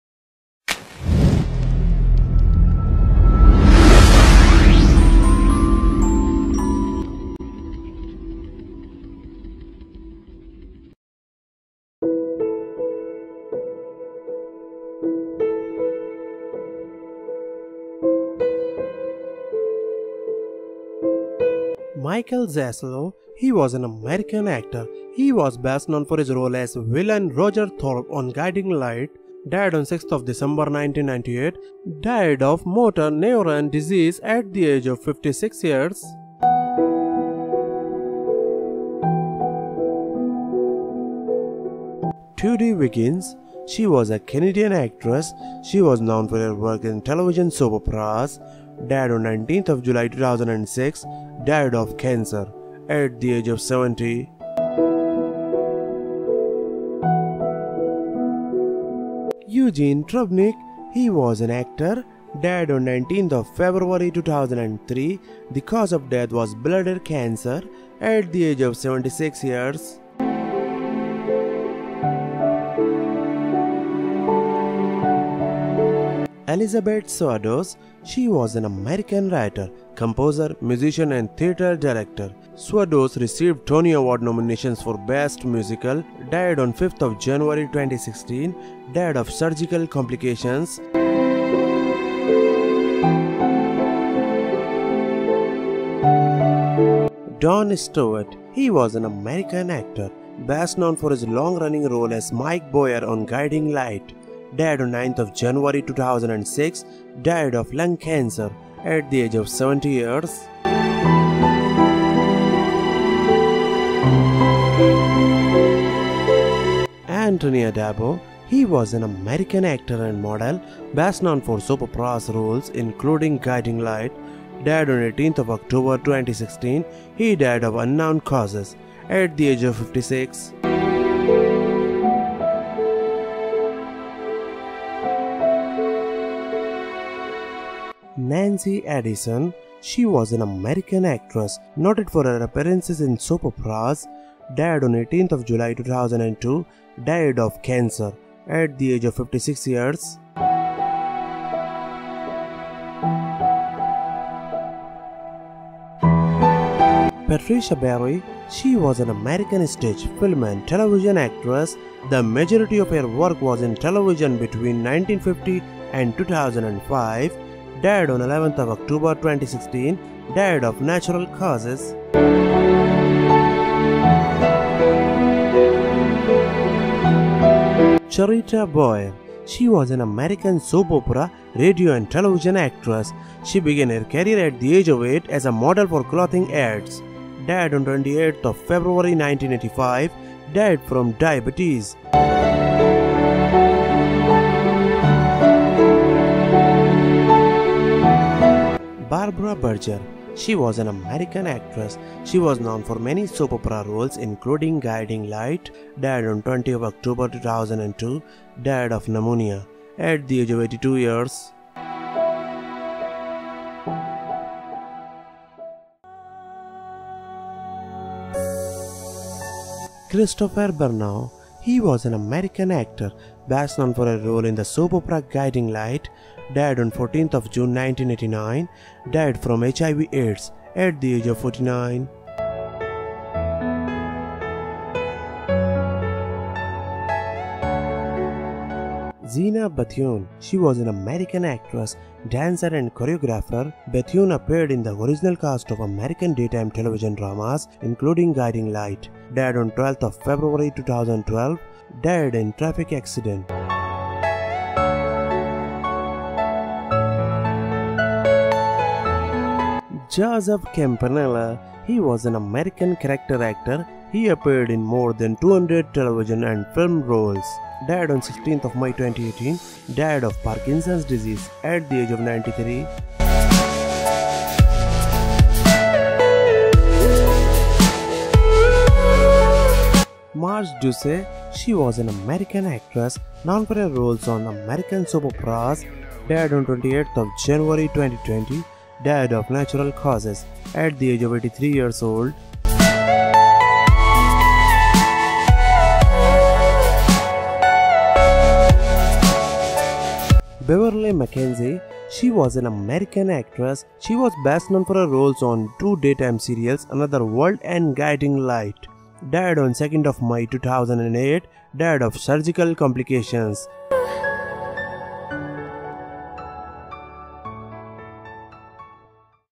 Guiding Light Cast Who Have Died || Soap Opera Deaths
Guiding Light is an American radio and television soap opera of CBS television. In this video i will showing Guiding Light cast who have died, deaths of soap opera Guiding Light with Photos, cause of death, date of death and much more information about Guiding Light cast.

It is listed in Guinness World Records as the longest-running drama in television History, broadcast on CBS for 57 years from 1952 to 2009. I have watch some of Guiding light full episodes on daytime.